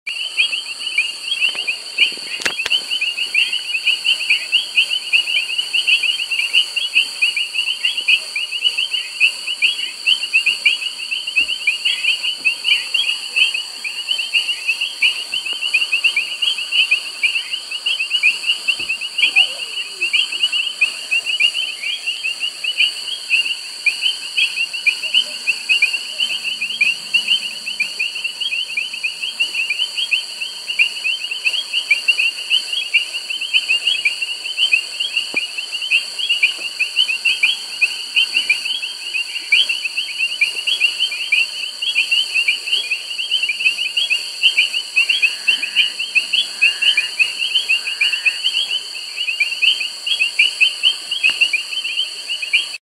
Relaxing Night Live Recording
This recording was made with my phone late night... Ive passed a bridge over a mountain stream and I couldnt resist... Tzaneen - South Africa